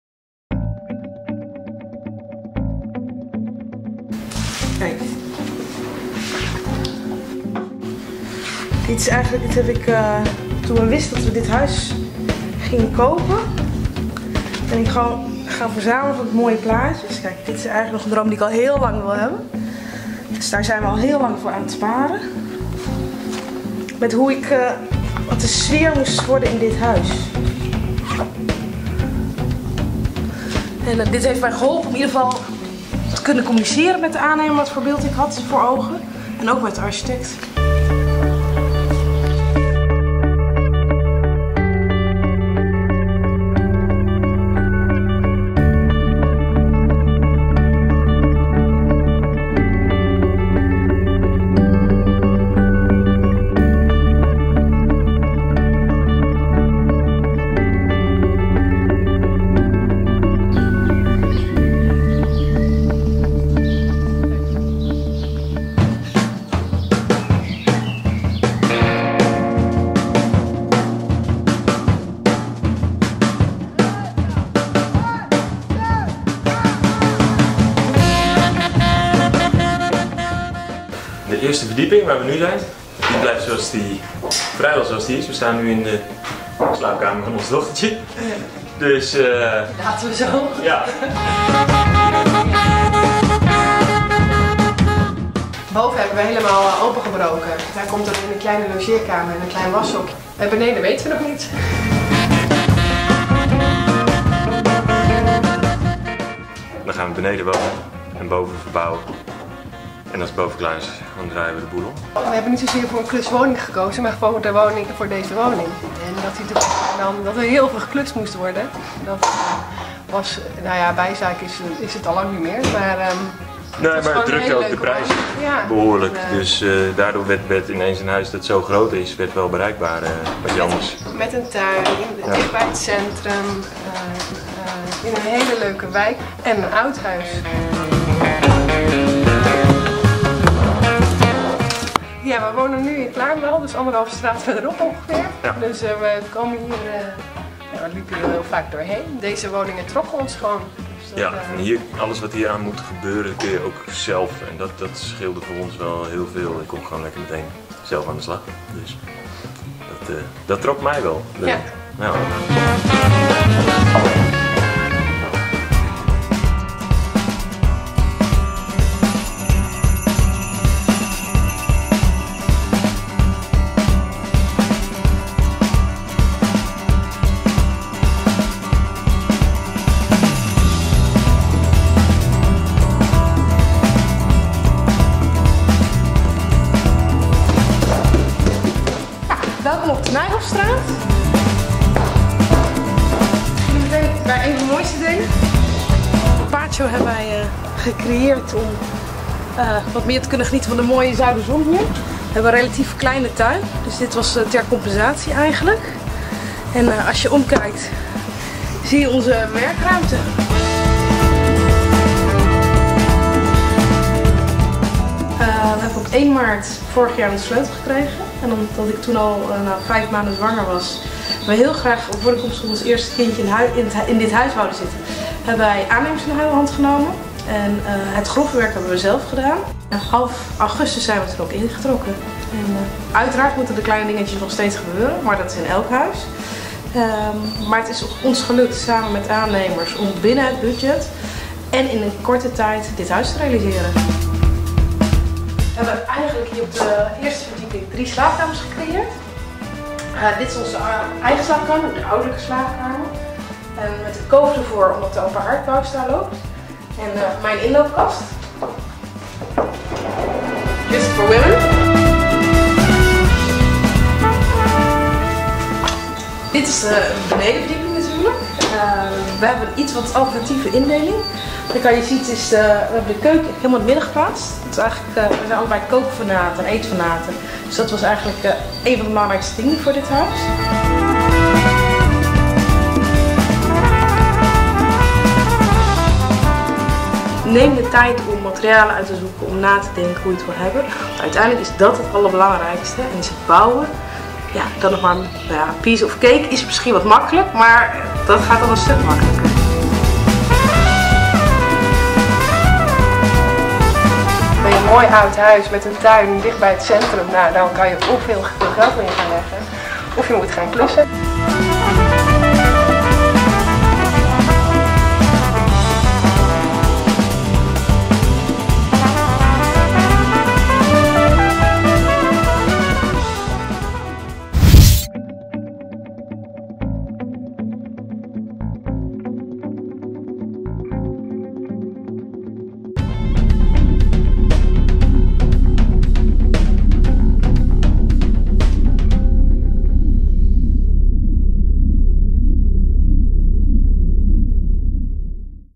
Klusomentary
Meekijken in de klushuizen in de Nijhoffstraat in Arnhem: Esther, Gerrit, Liesette en Abner laten zien hoe ze hun nieuwe woningen aanpakken
Een mini-doc door Ids Breeuwsma van Portaal Arnhem